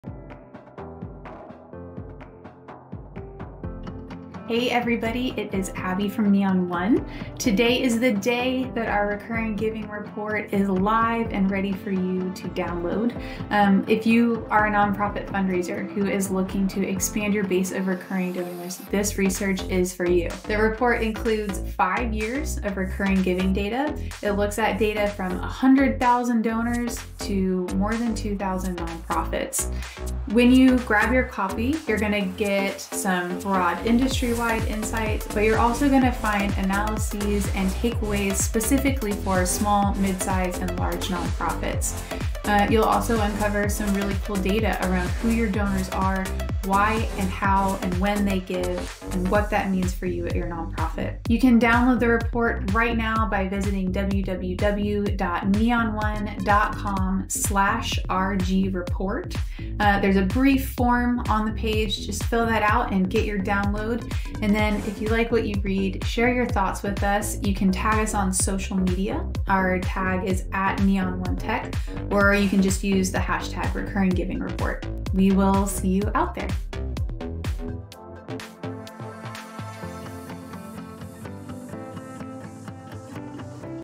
Neon One's Recurring Giving Report -- Available Now!
Individual giving is down. Donor retention rates remain low. Reliance on major donors is on the rise. But recurring donors—people who commit to supporting their favorite causes on an ongoing basis— are a bright spot in our sector. These highly engaged supporters are a reliable, predictable source of support for the organizations they love.

Today, we're proud to release The RecurringGivingReport. Download the report now for a look at how recurring donors supported their favorite organizations between 2018 and 2022. You'll find breakdowns of important benchmarks, insight into when and how these donors give, and expert advice for anyone looking to expand their recurring donor base.

, and let us know your thoughts using the hashtag RecurringGivingReport.